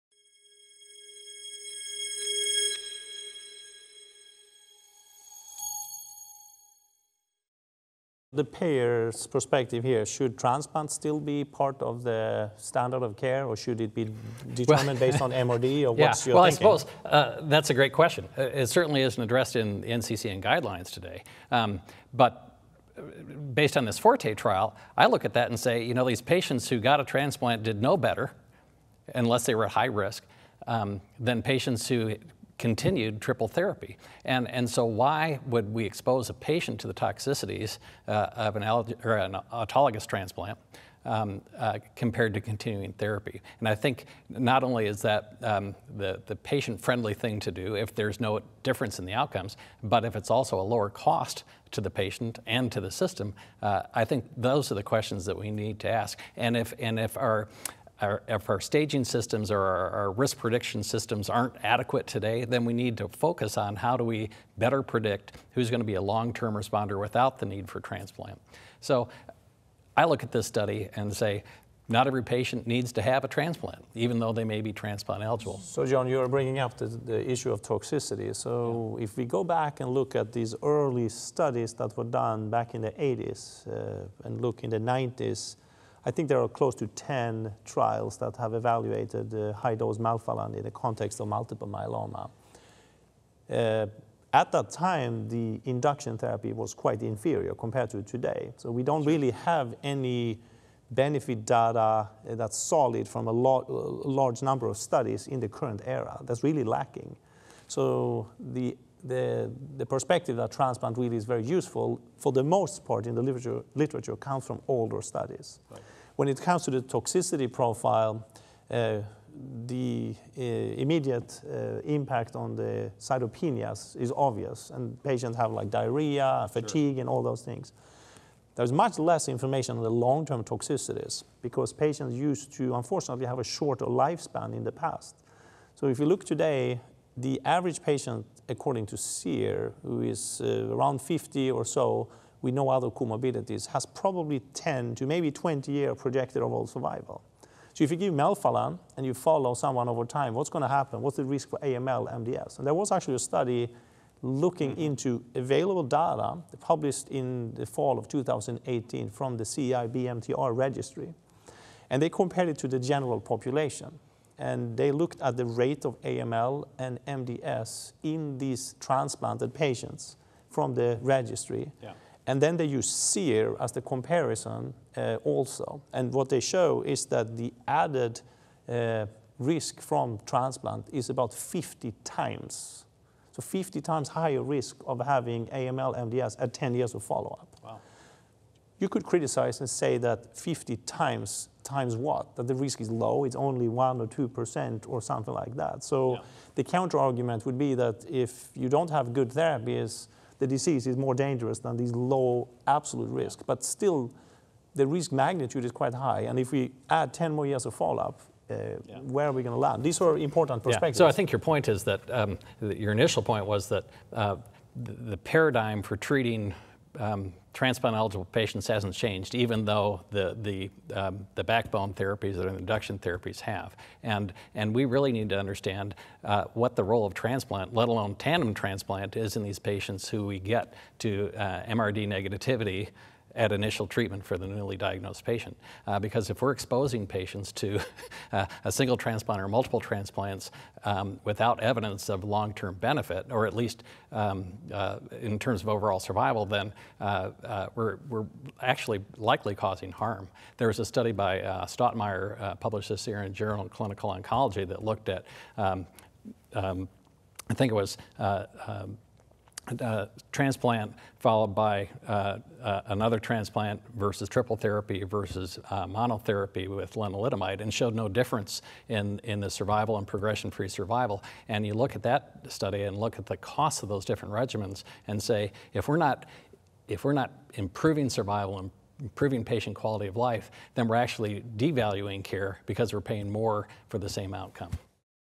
Payer Perspective: Transplant and Toxicities in MM Therapy
John Fox, MD, MHA, provides a cost perspective on transplant as a standard of care and discusses associated toxicities in patients with multiple myeloma.